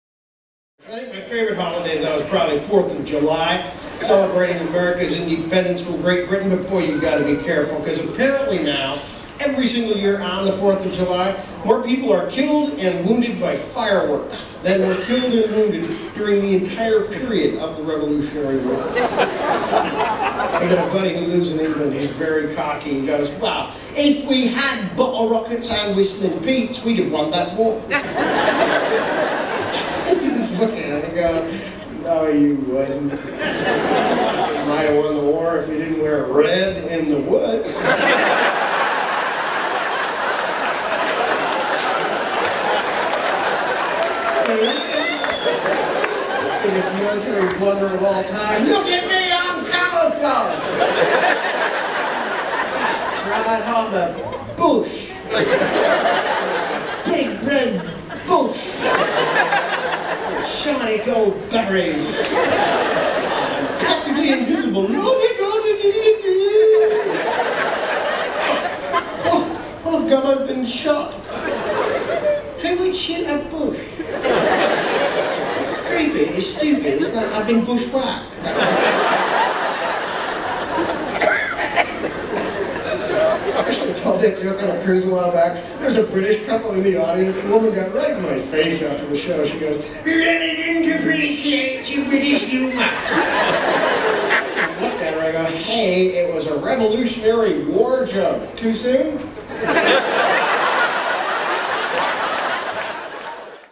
Tom McTigue - Fourth of July-web.mov
Stand up comedian Tom McTigue's classic take on the Fourth of July - HILARIOUS!